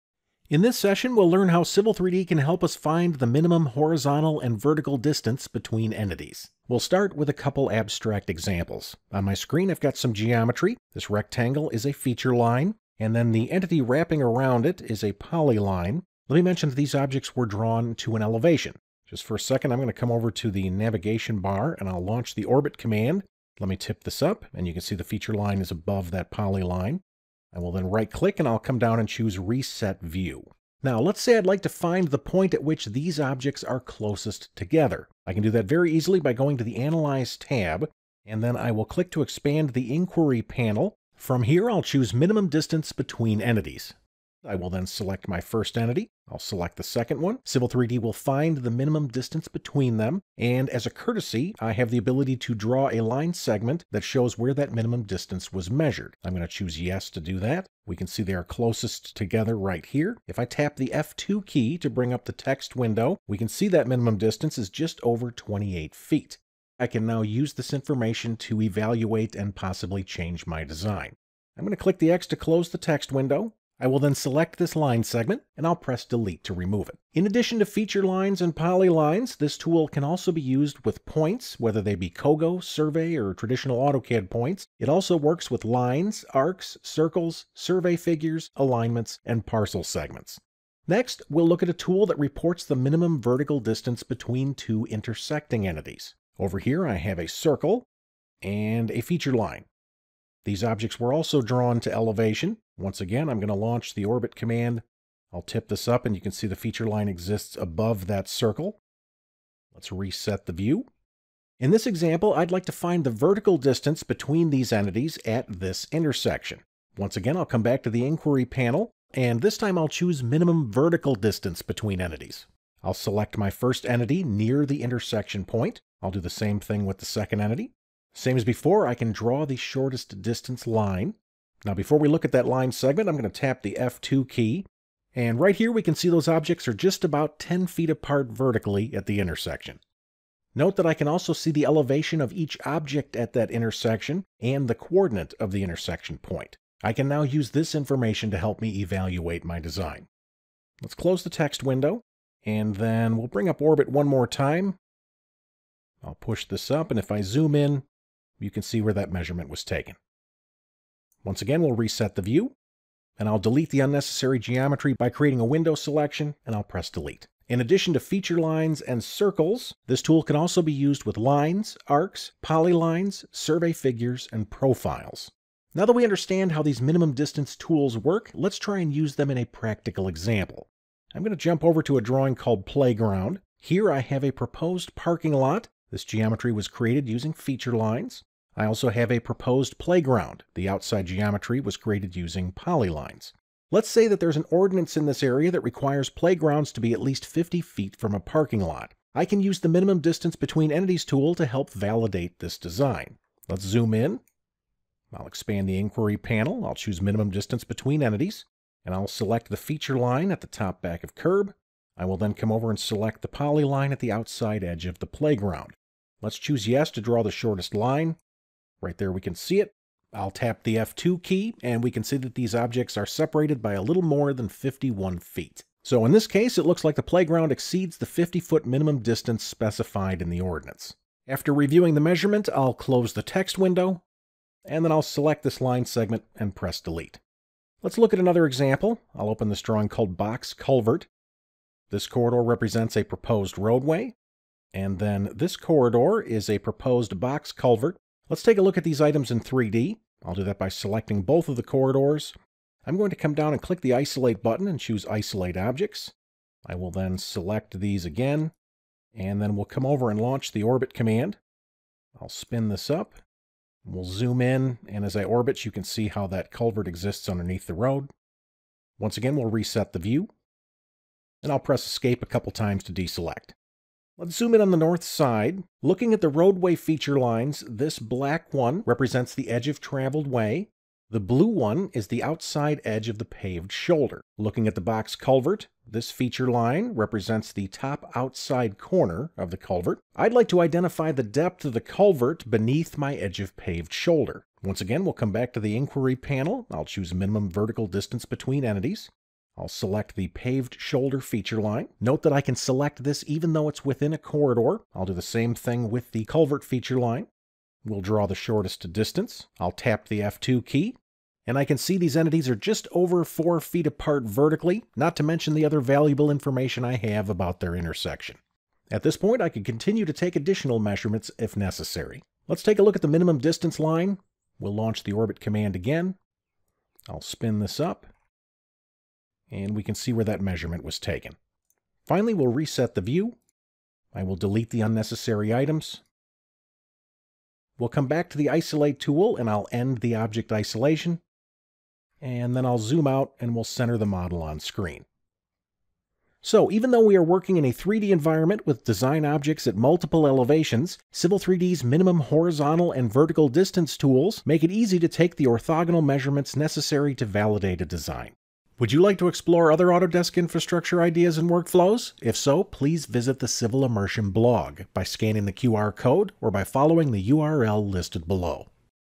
Finding the Minimum Horizontal & Vertical Distance Between Entities using Civil 3D
Since Civil 3D represents a three dimensional design environment, the entities in a civil model can exist at multiple (or varying) elevations.  Fortunately, Civil 3D makes it easy to take the orthogonal measurements necessary to validate a design.  Today we will explore some workflows using the “Minimum Distance Between Entities”, and the “Minimum Vertical Distance Between Entities” commands.